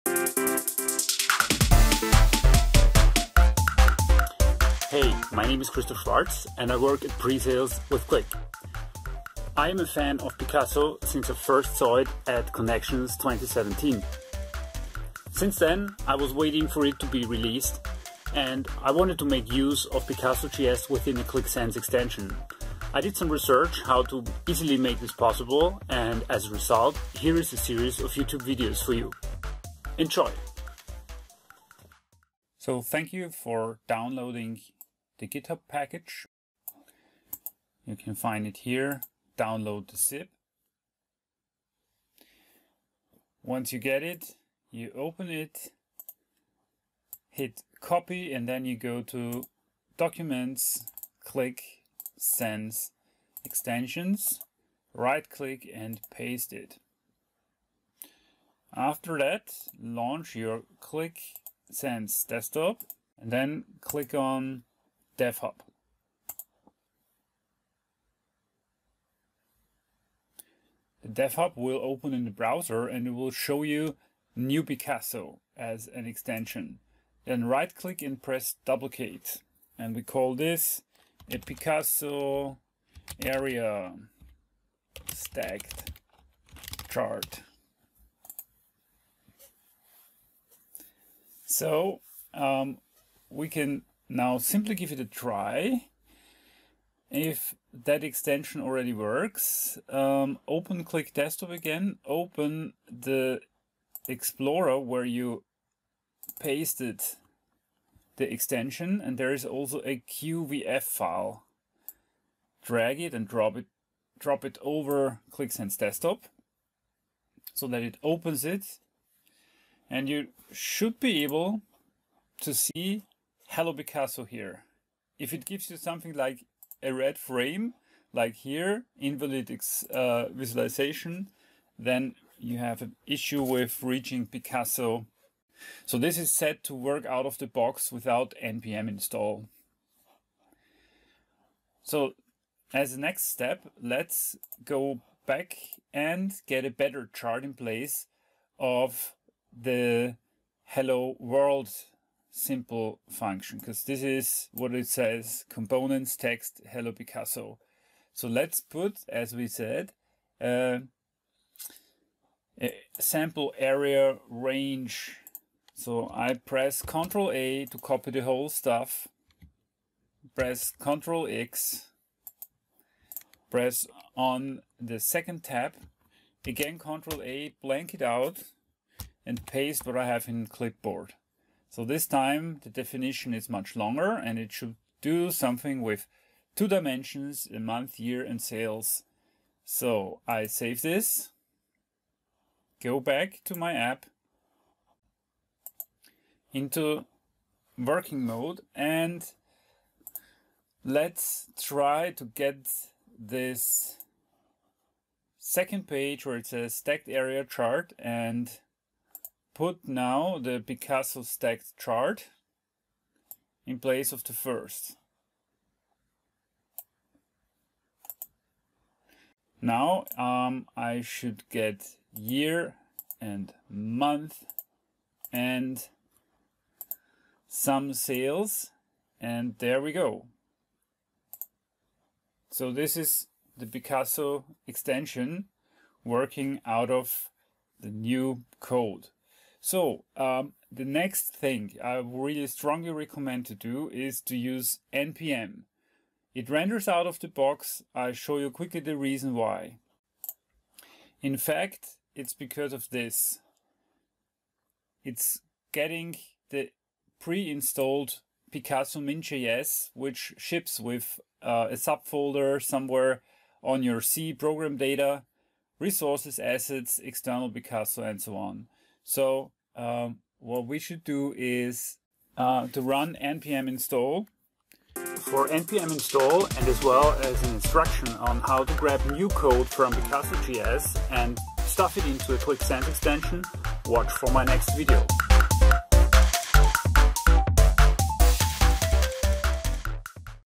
School of Picasso JS Part 1: Get started
and get started with Picasso.js inside of a Qlik Sense extension. Attention: The video was filmed with a previous version, where you had to "Duplicate" the downloaded extension. The new one installs as a "Extension Template" and you get it by clicking "New Extension" in the /dev-hub